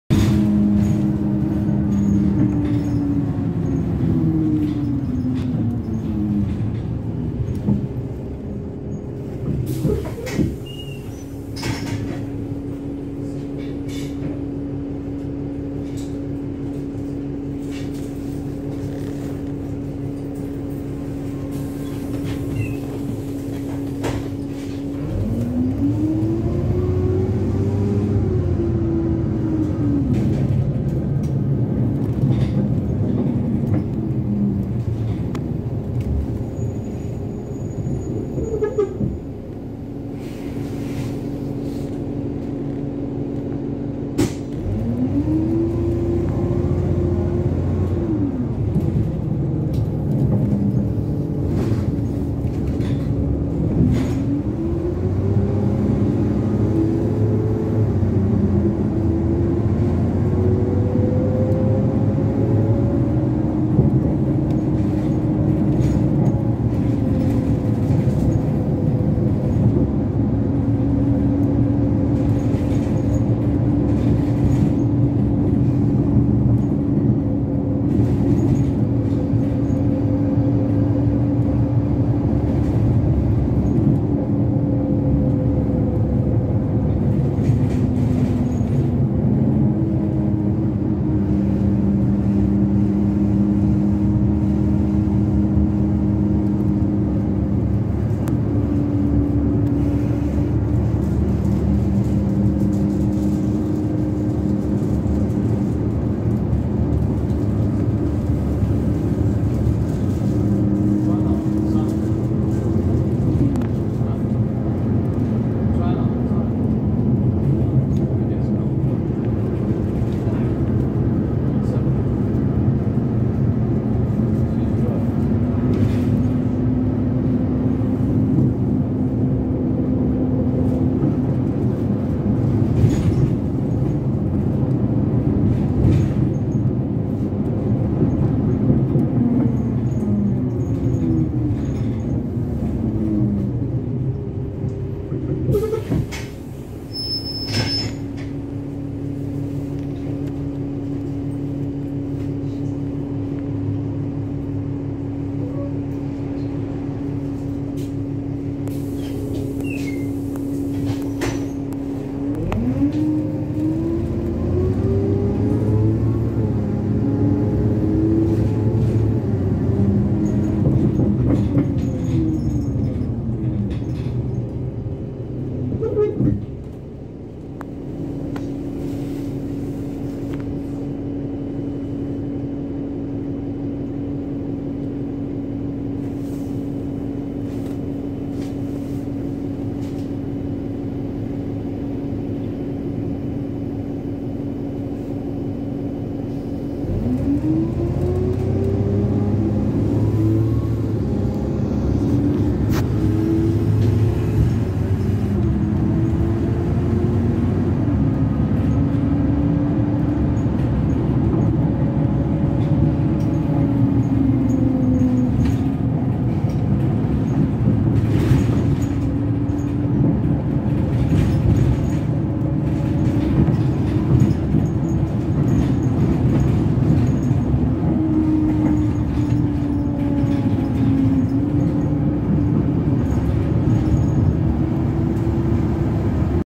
(THRASH!!) Ex Yellow Buses Optare Versa [21] [T21 TYB]
Sry for the weird angle :D, also it was making them crunching noises probably cause of the REALLY fat person in the back